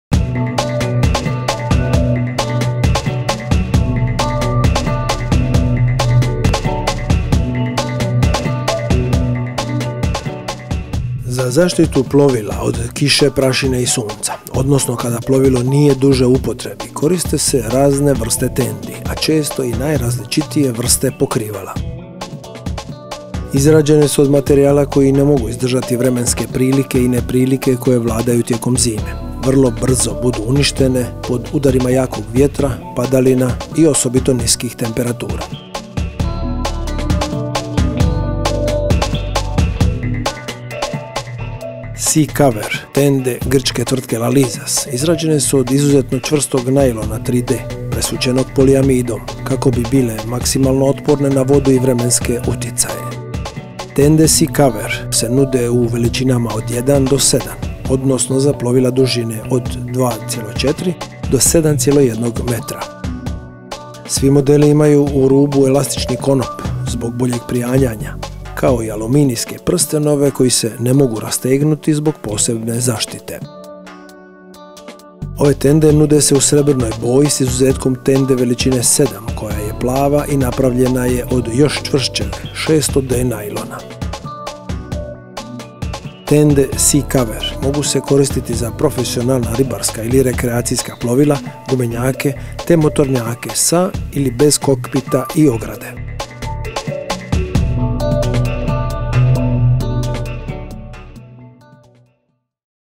Cerada za Brod
Za zaštitu plovila od kiše i sunca te kada plovilo nije u upotrebi koriste se razne vrste tendi.